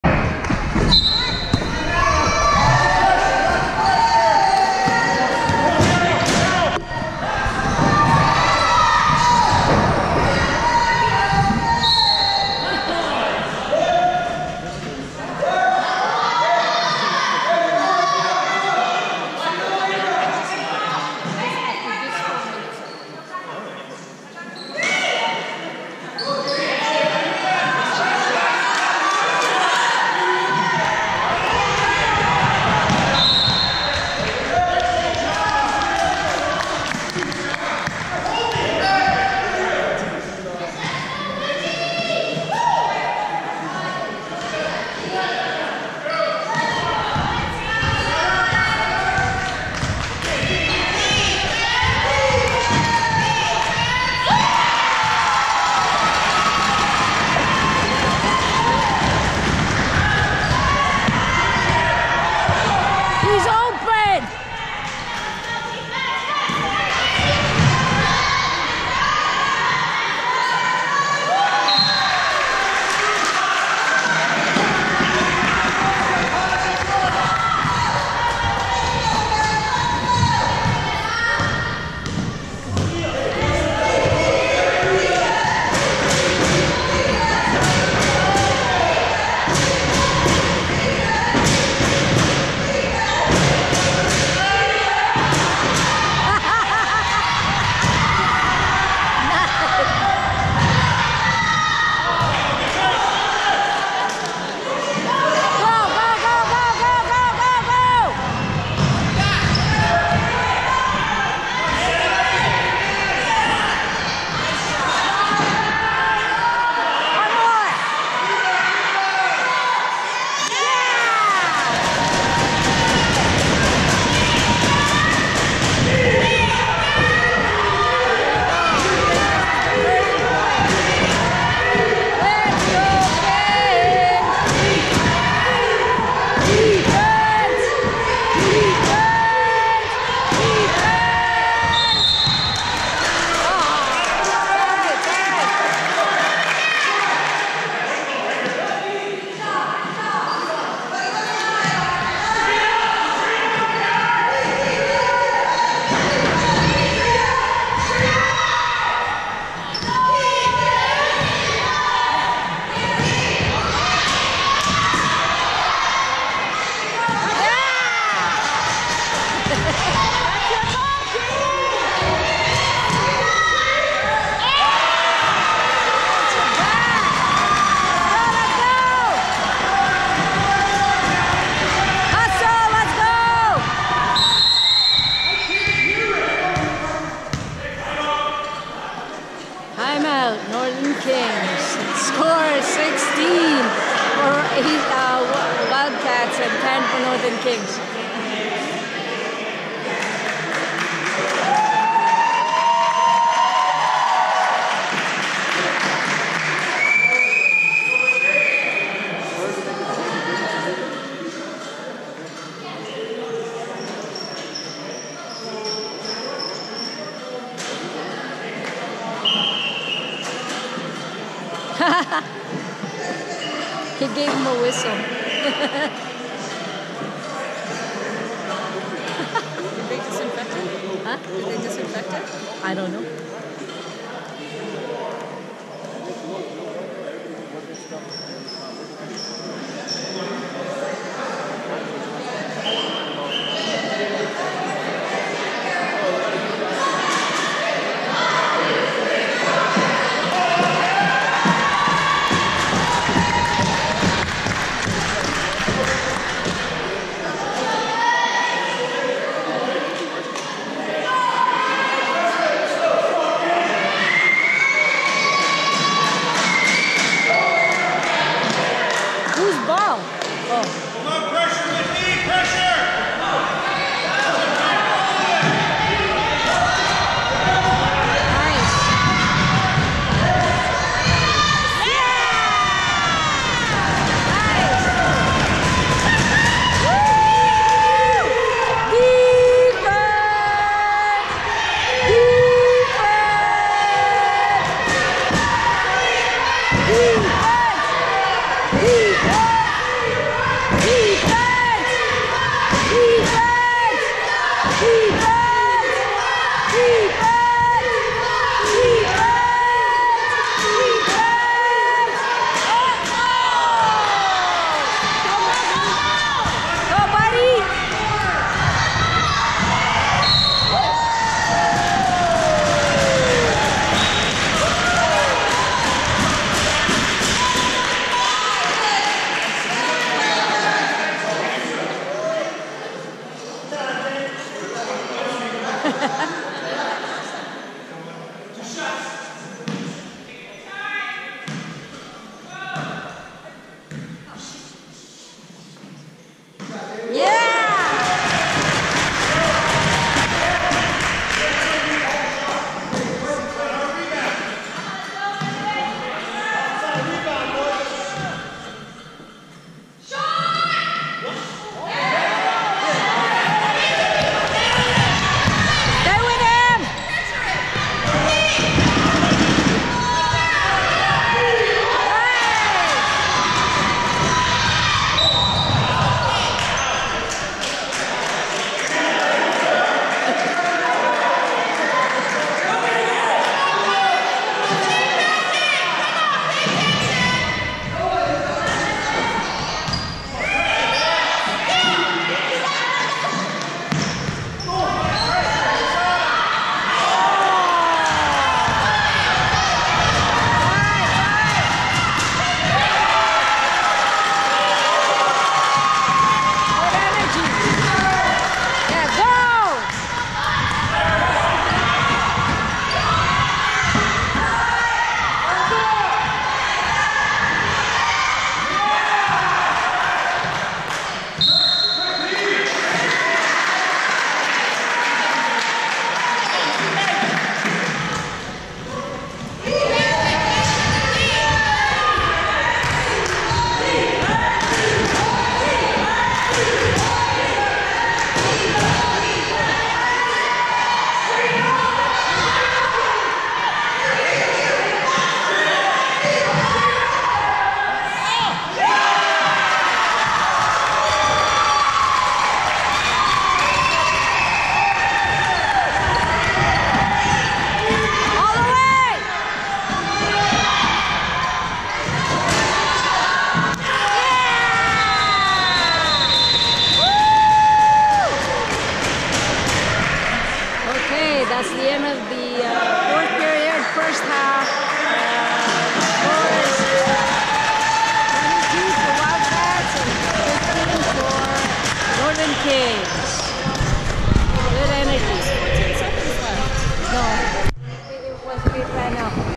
MAR 30.2019 NORTHERN KINGS EAST VS WHITBY WILDCATS - 4th Period-First Half
OBL PROVINCIAL TOURNAMENT MAR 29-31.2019
JEANNE SAUVE FRENCH IMMERSION P.S.,  ST. CATHERINES, ON